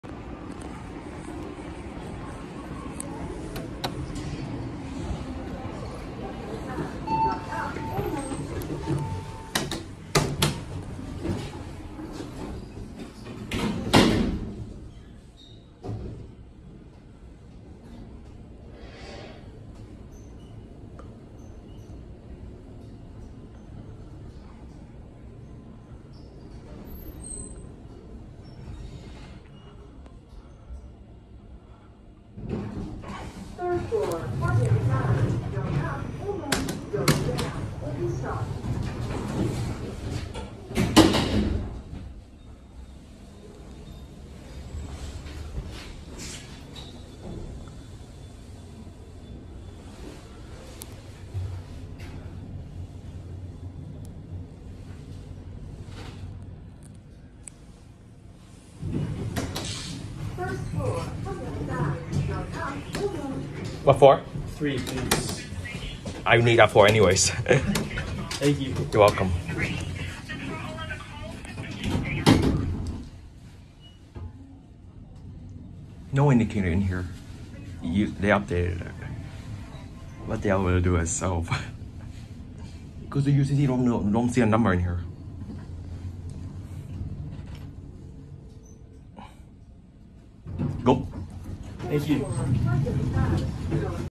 Otis Traction Elevators - Toronto Pearson Airport (Terminal 3), Mississauga ON
These elevator normally are the busiest in this terminal but turn out it not when I came to these elevator. The elevator haven’t change since last take of these. The TV screen where the use as floor position indication inside the elevator did so update but forgot to put it. Used to be Otis with Older Series 1 and got first mod by ThyssenKrupp Northern and got pointless second mod maybe by ThyssenKrupp or Modern a Canadian elevator company did it.